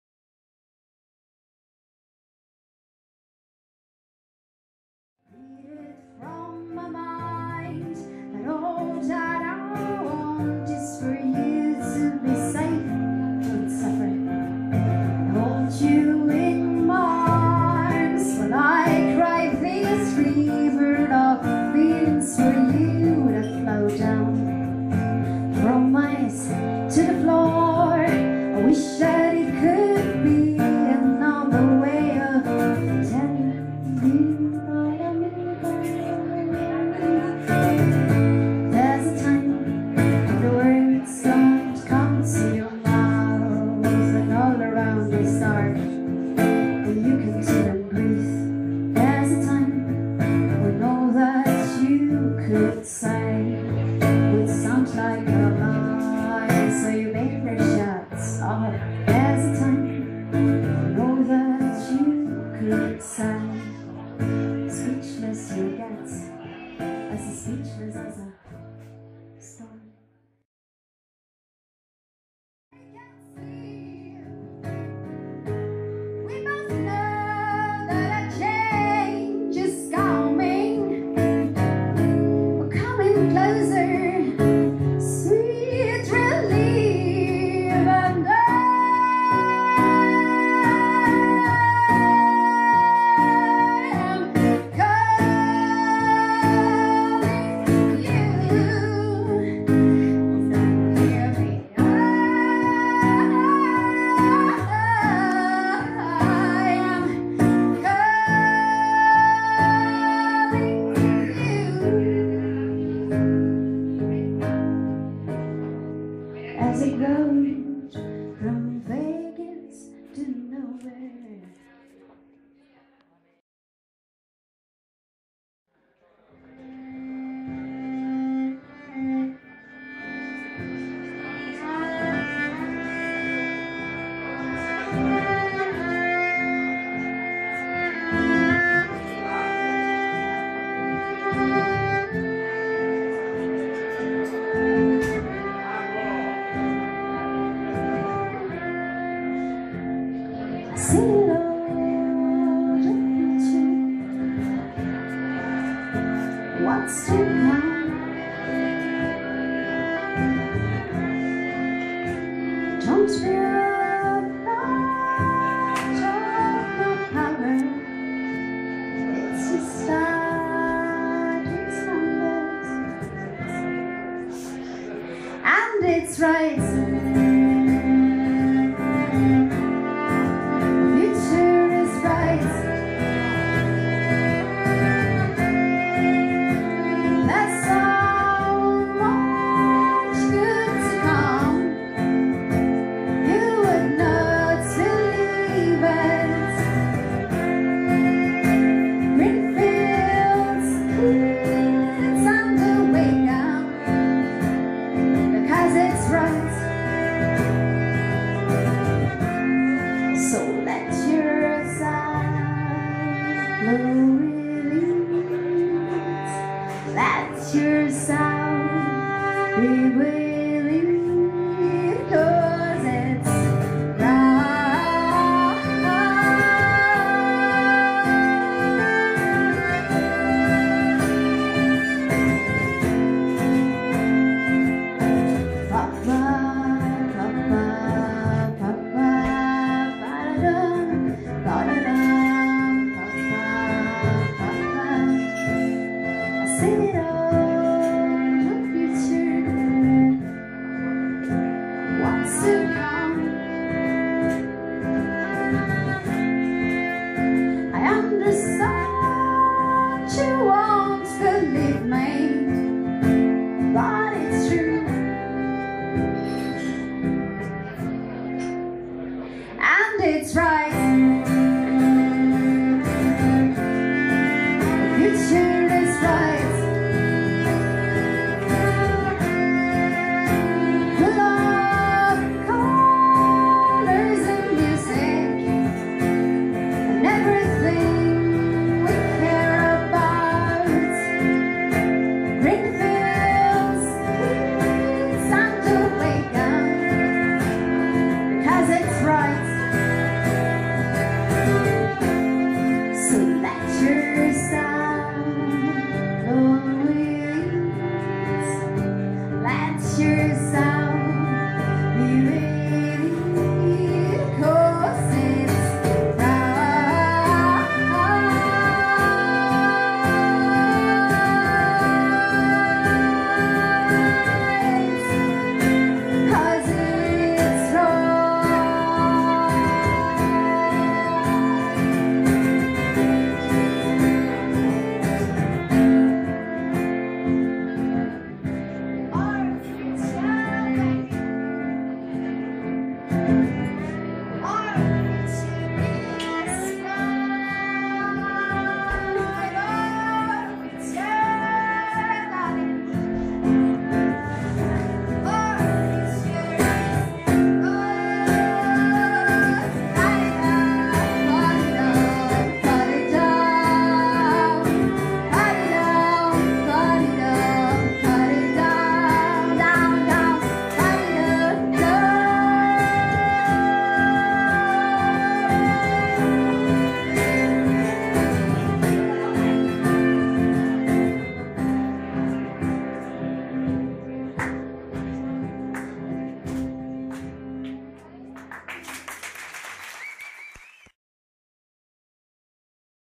Mehnai@El Cazurro
concierto acústico de Mehnai en El Cazurro, La Arnía (Cantabria) 28 de Junio de 2012